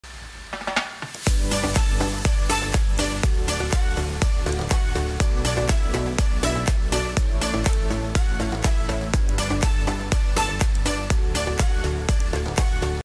Electric Candle without Fuel
Tesla coil electric candle operates with high voltage at 400V to produce arc flame with air and molten particle of the screw and create the plasma.